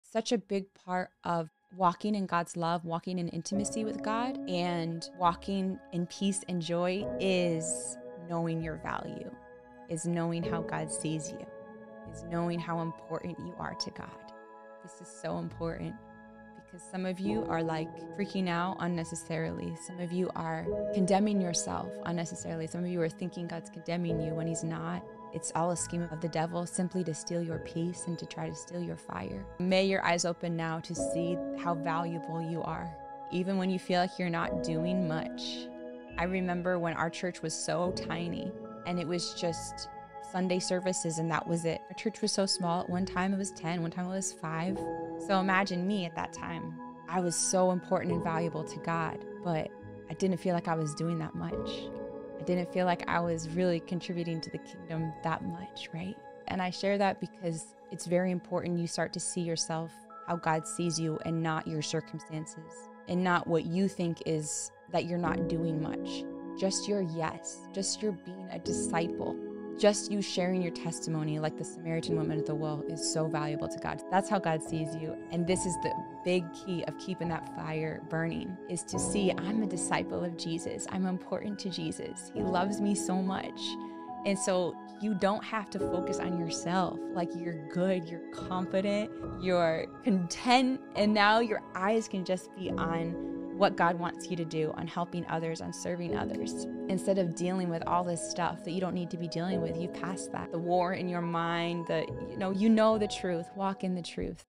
The Importance of Seeing Your Value
The enemy wants to fill you with condemning thoughts and lies about your identity so you’ll be so distracted with negative thoughts about yourself that you don’t have time or energy to focus on others and be effective in the kingdom. Just your yes to God, just your simple obedience makes you SO VALUABLE in the kingdom of God! Stop falling for this scheme so you can walk in contentment and confidence as a child of God and so you can do powerful things with Jesus for His Kingdom! 🔥